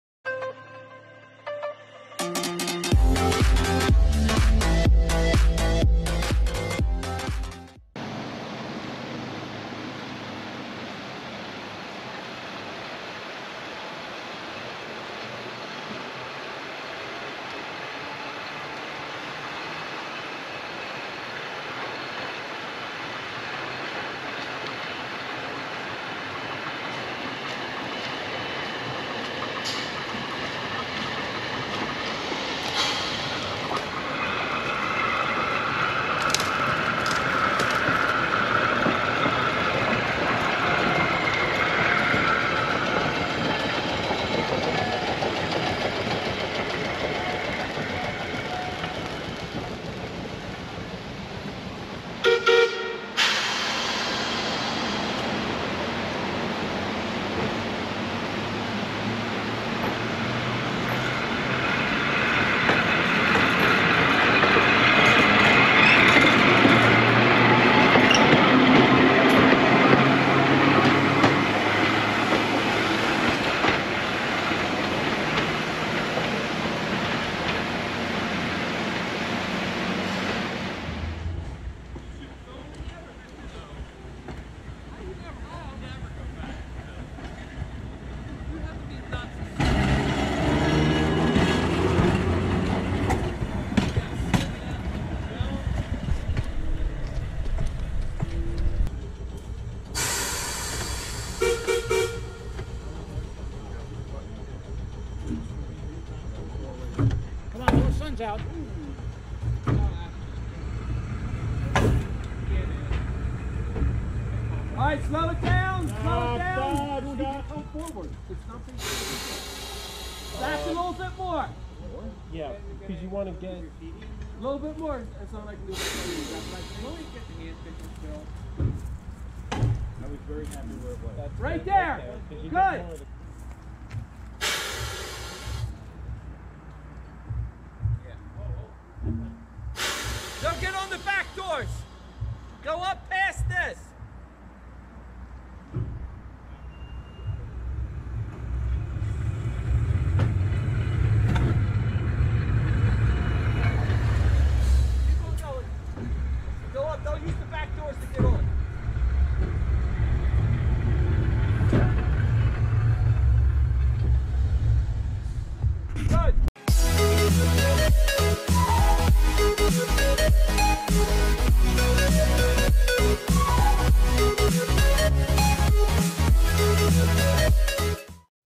Electric City Trolley Museum Collection - Episode 1 (FPT Charter)
Coast to Coast Electric Rail Content

The Electric City Museum acquired P&W car 164 in November of 2009 from the East Troy Electric Railroad, this is why the car is still lettered for East Troy. Car 164 is seen in inside Crown Avenue Tunnel beneath Scranton, Pennsylvania on September 25, 2021. Car 164 was built by the J.G. Brill Company in 1927. Car was on a FPT photo trip.